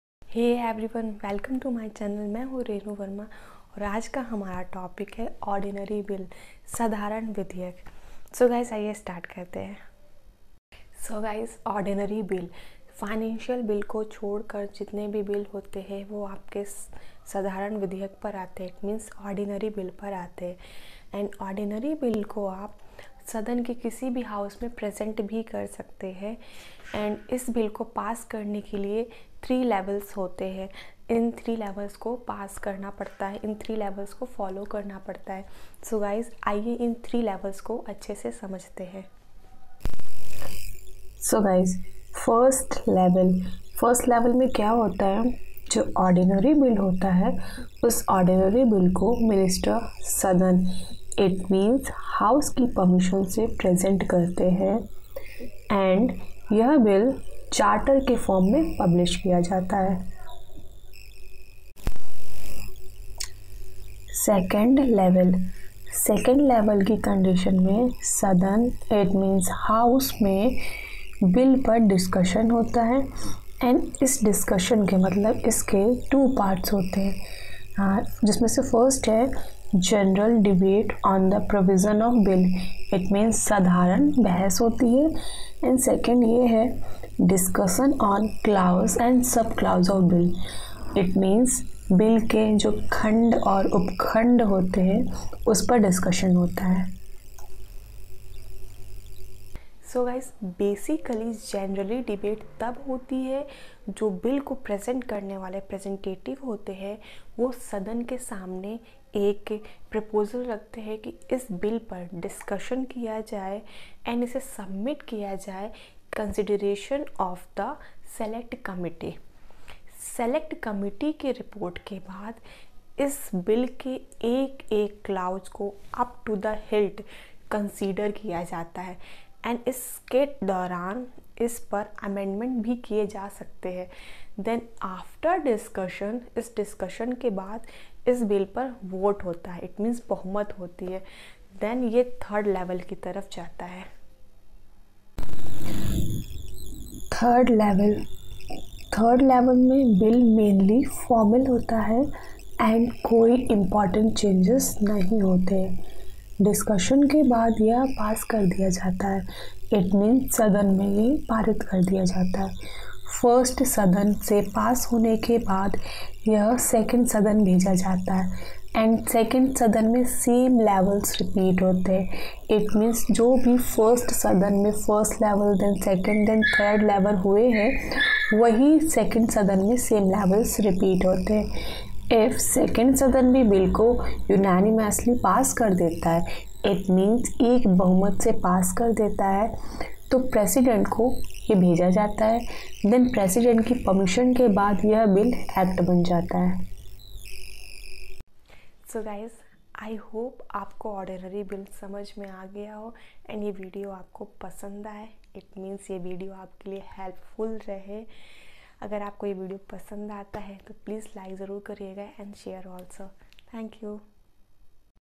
WHAT IS ORDINARY BILL? WHAT IS THREE LEVEL OF ORDINARY BILL?
Hello dosto.

ORDINARY BILL KYA HOTA HAI?
ISKE THREE LEVELS KYA HOTE HAI..?
If you haven't seen my previous video So you link is given below.

WHAT IS BILL ?
TYPES OF BILL...
THANKYOU